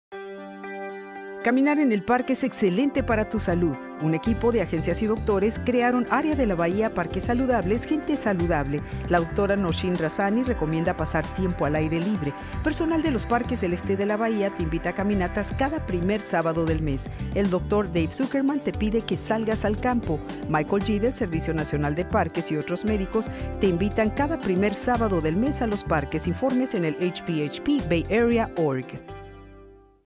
En Espanol/In Spanish: Get Outdoors Video
Information about the East Bay Regional Park District - in Spanish.
Disfrute del aire libre en los parques regionales.